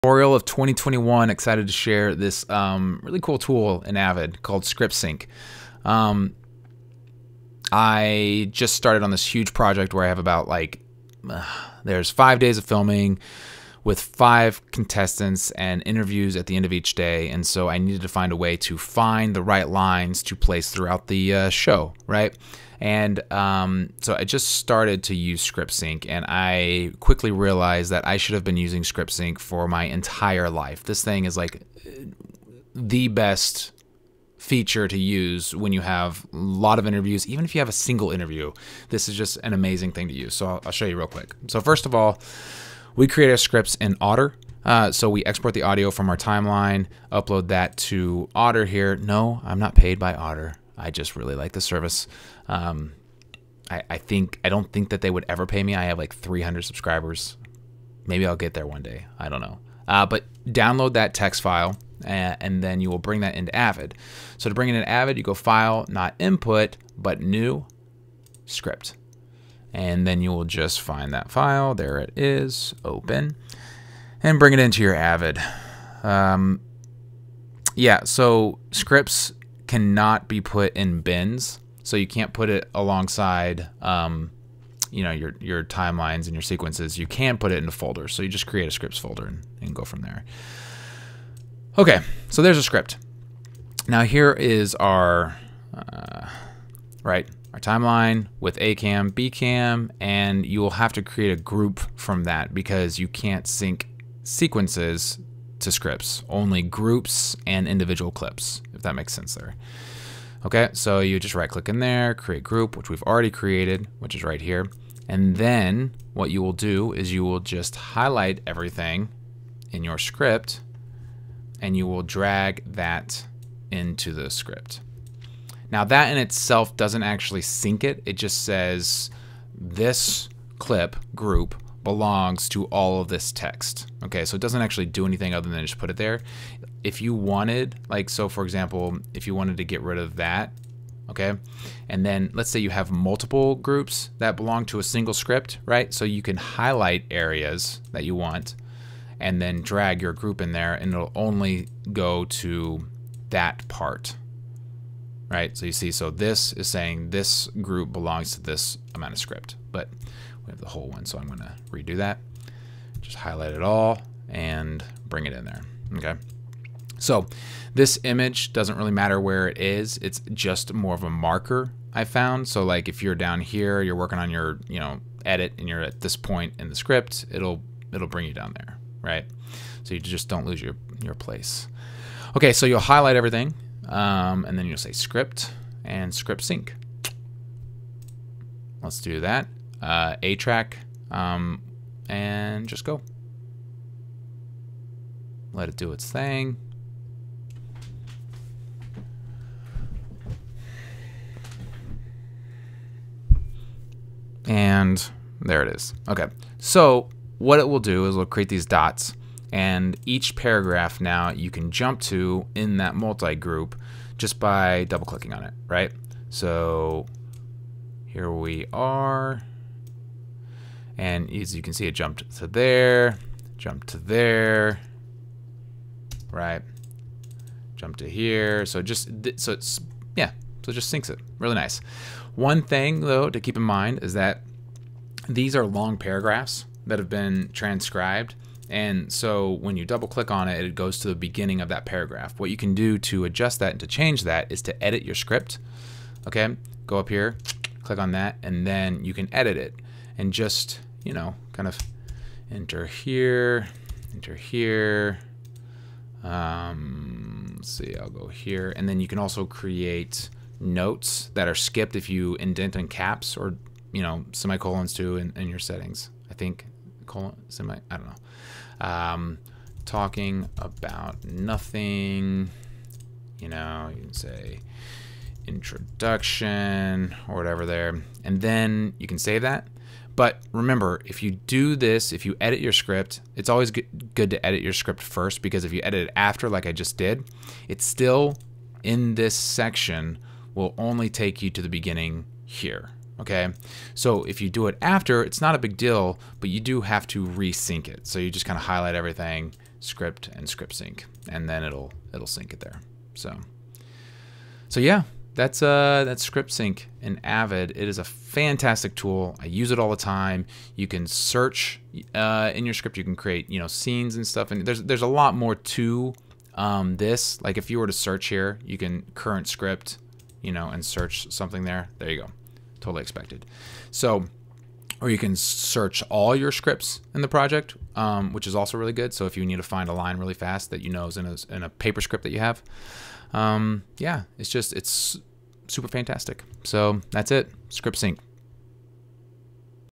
Script Sync - AVID MEDIA COMPOSER
Syncing scripts to your interviews in Avid Media Composer.